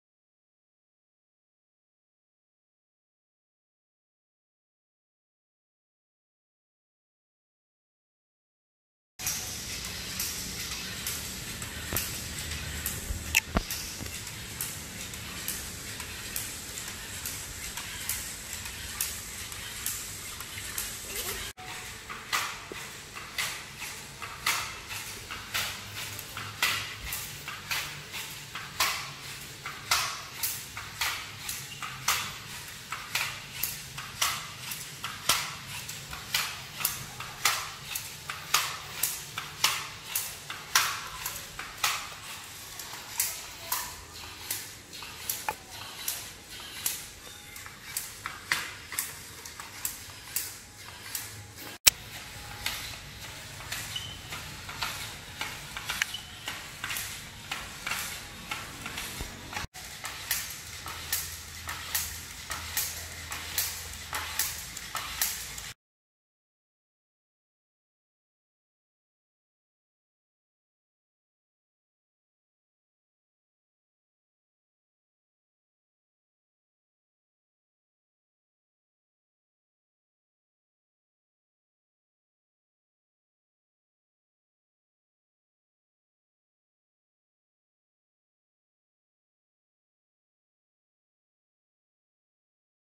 LEGO MINDSTORMS EV3 "ALLIGATOR"
Hello guys! In this video I'll show you my EV3  Alligator!